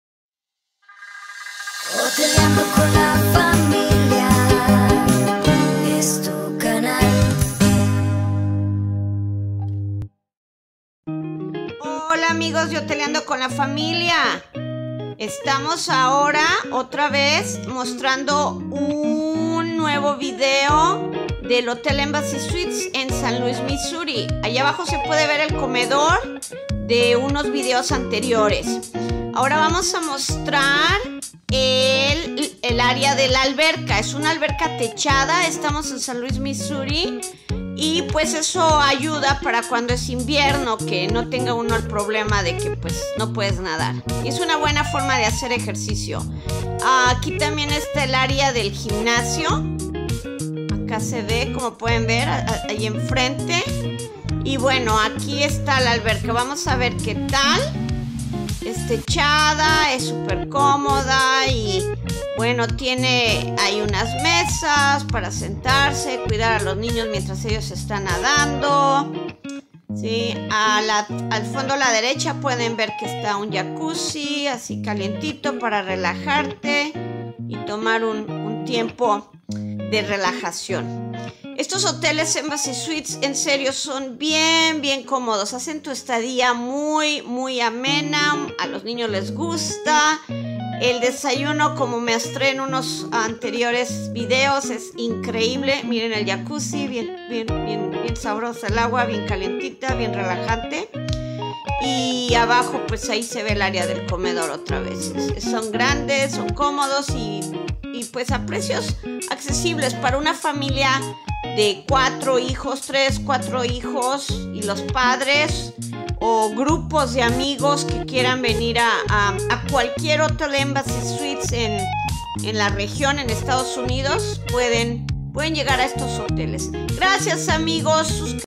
Alberca en Embassy Suites Saint Louis Missouri.
Hola amigos de Hoteleando con la familia. Está alberca y jacuzzi techadas se encuentran en el hotel Embassy Suites en Saint Louis MO.
Hello friends of Hoteleando con la Familia. These indoor swimming pool and jacuzzi are some of the attractions of The Embassy Suites Hotel in Saint Louis Missouri. Created by InShot